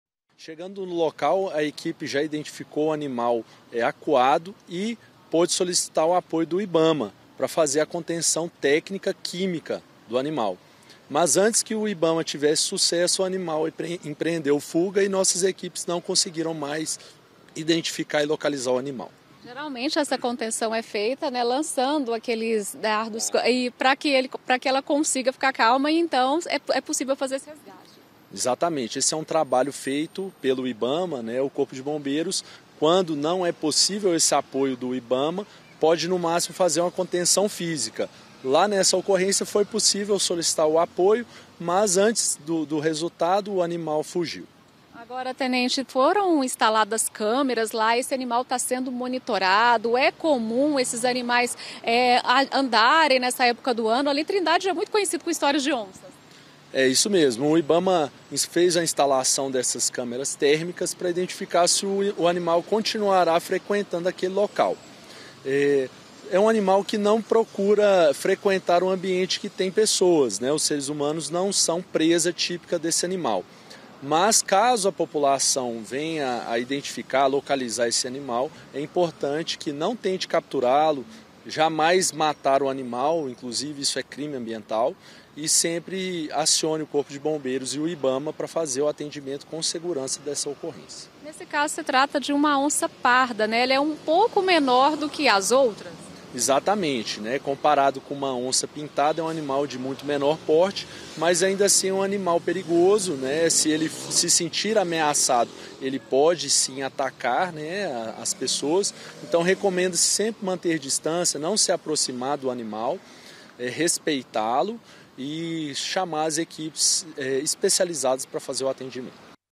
CADÊ A ONÇA: O IBAMA SEGUE MONITORANDO A ÁREA ONDE UMA ONÇA PARDA APARECEU NA SEXTA-FEIRA.
O ANIMAL FOI FLAGRADO DESCANSANDO EM UM PESQUE E PAGUE DE TRINDADE. VAMOS ASSISNTIR UMA ENTREVISTA COM MAIS DETALHES SOBRE ESSA OPERAÇÃO.